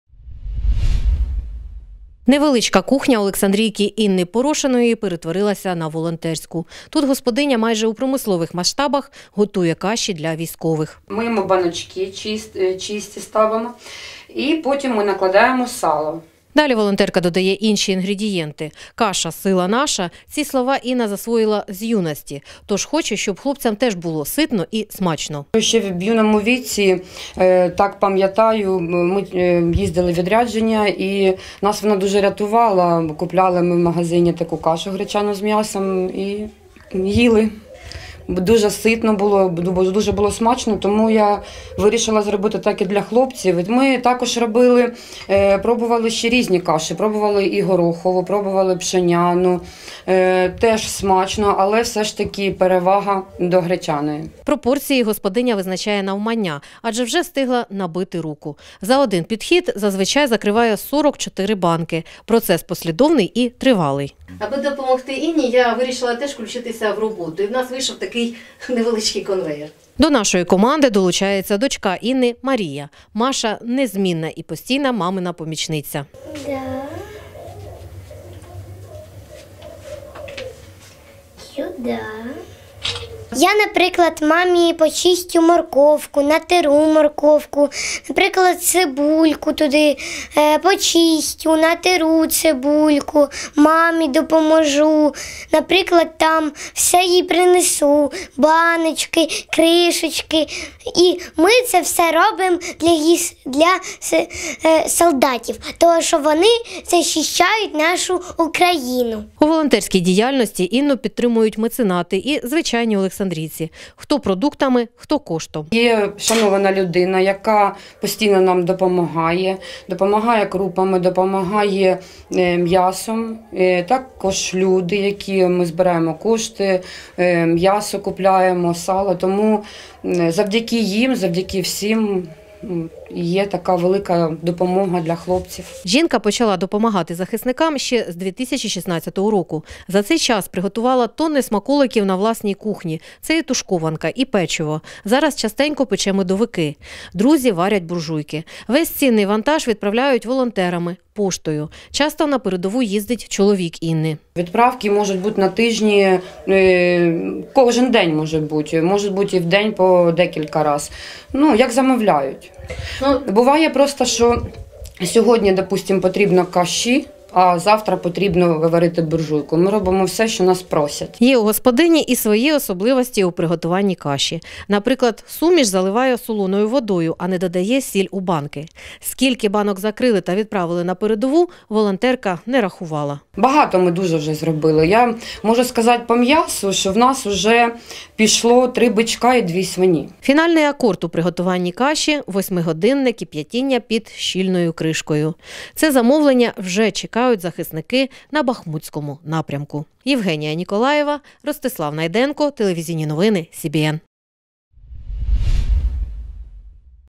Олександрійка в промислових масштабах готує каші для військових | телеканал Вітер
“Каша - сила наша” - під таким гаслом олександрійка Інна Порошина готує ситний і смачний продукт для фронту. Крім того, жінка випікає печиво, торти, робить тушкованку. На волонтерській кухні побувала наша знімальна група.